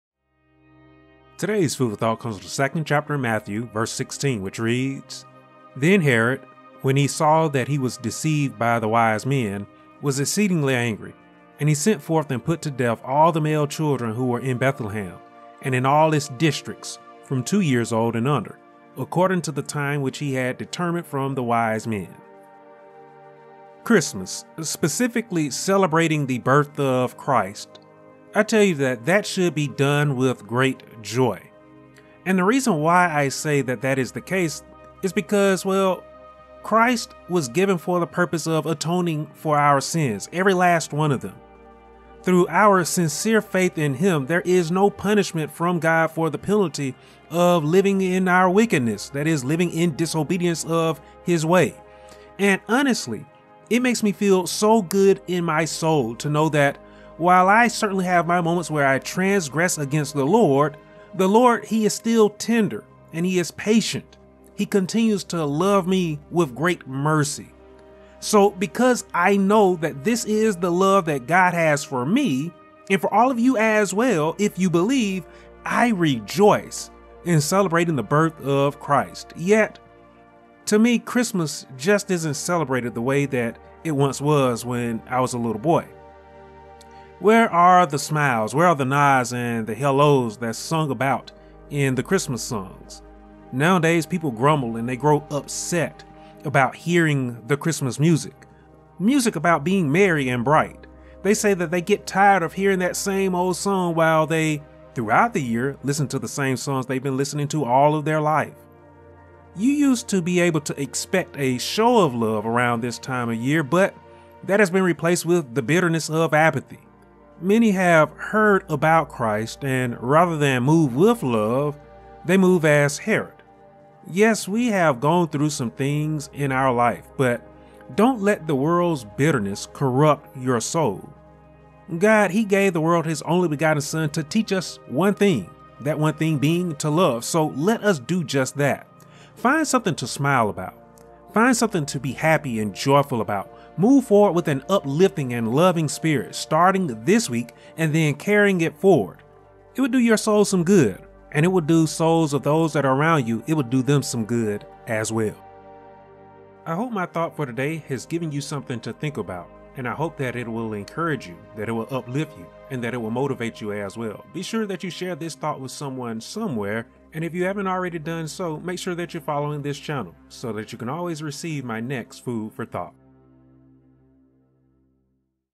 Where’s Your Joy | Matt. 2:16 | Food For Thought
Yes, life can be tough on us but we are God's children, why let the world make you bitter in your heart?

Podcast locations
Amazon music